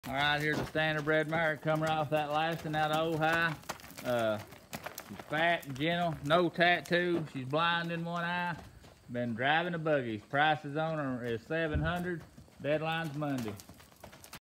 Sold Black standardbred mare
Status : ‼️AVAILABLE ‼️
Tag Number: no tag
Description: 15 yr old one eye
Price: $ 700
Deadline: 10/4/21